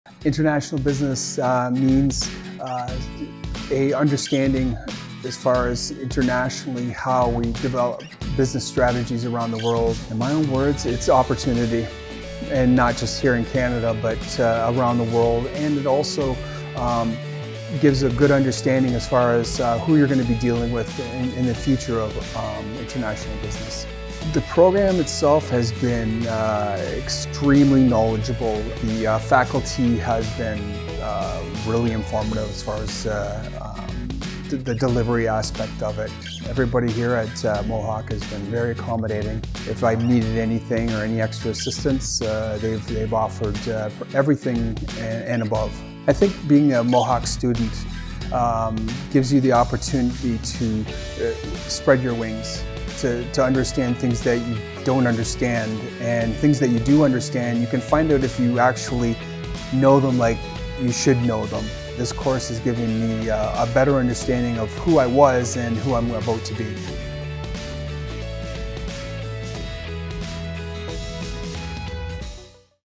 International Business Management - Product Management Graduate Certificate
Learn how to drive the success of a business within local and international environments. Support the research and development of products and services in alignment with strategic business goals.